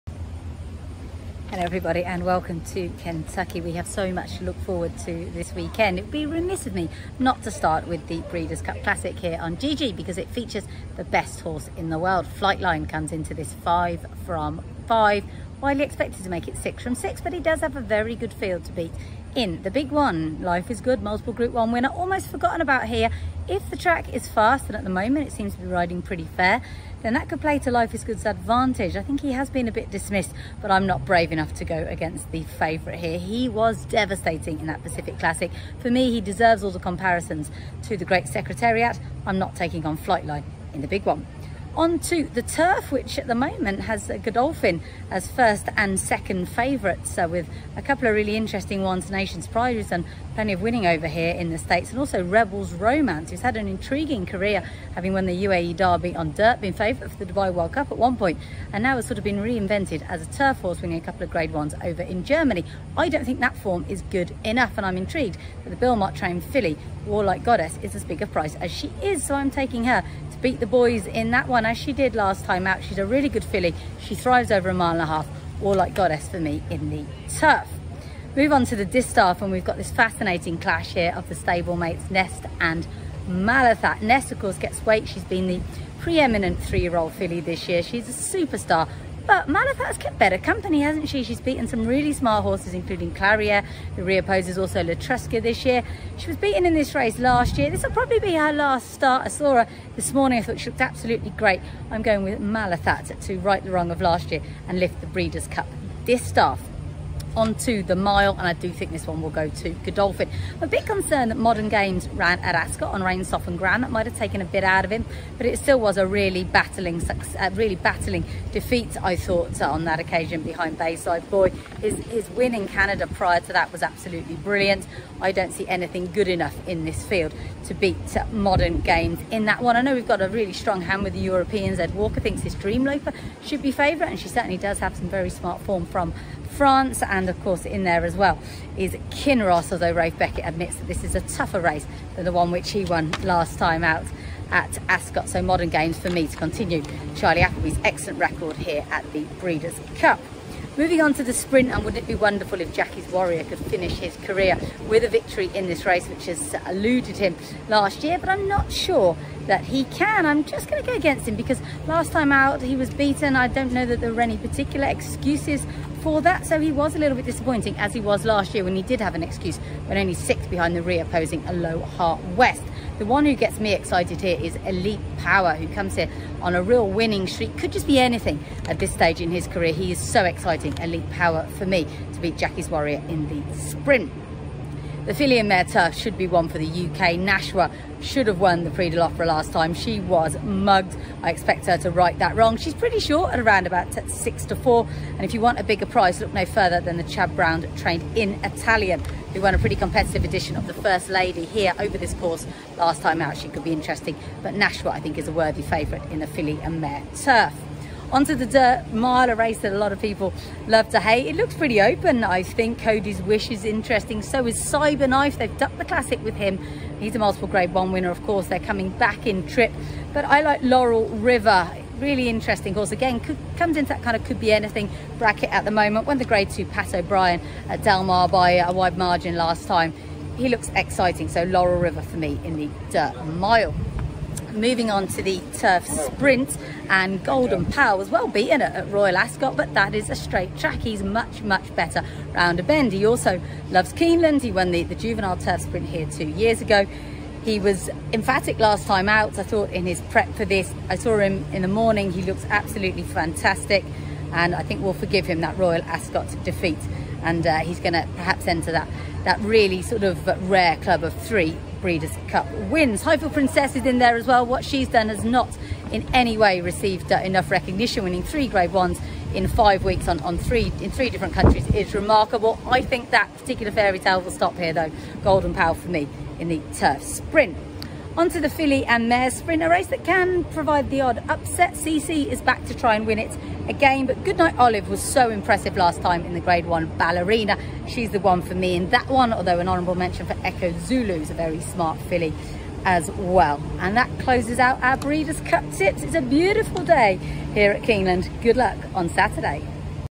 Breeders' Cup Saturday Tips | Laura King's International View | 5th November 2022
Day Two of the Breeders' Cup promises plenty more world class racing at Keeneland, and Laura King is lucky enough out there to see it all up close!

Our International racing expert talks us through Saturday's incredible card, where she's expecting Flighline to prove why he's the highest rated racehorse in the world.

Laura King’s Best Bets:
- Goodnight Olive (3.50)
- Golden Pal (4.29)
- Nashwa + In Italian (5.50)
- Elite Power (6.30)
- Modern Games (7.10)
- Malathaat (7.55)
- War Like Goddess (8.40)
- Flightline (9.40)